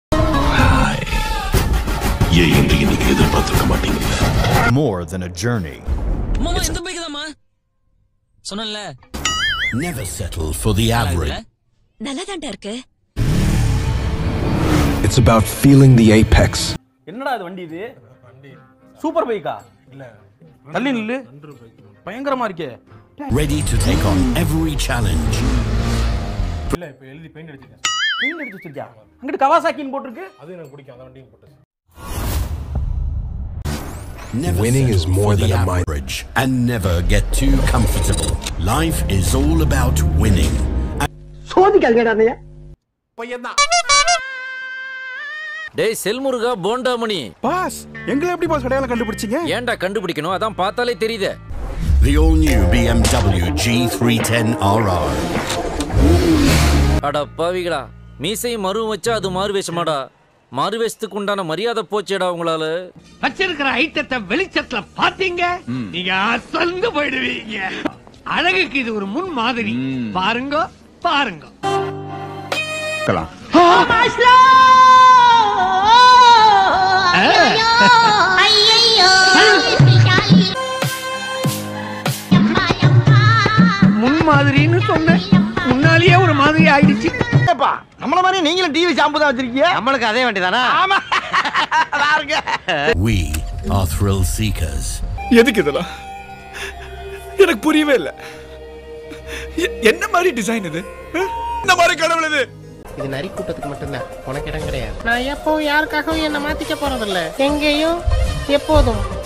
APACHE RR310 VS BMW G310 RR TROLL | Amazing All
welcome to Amazing All family

hai family members this video

APACHE RR310 VS BMW G310 RR TROLL

hai friends bmw new bike launched but APACHE rr 310 buying then changing logo

ready bmw new bike very funny launched g310 rr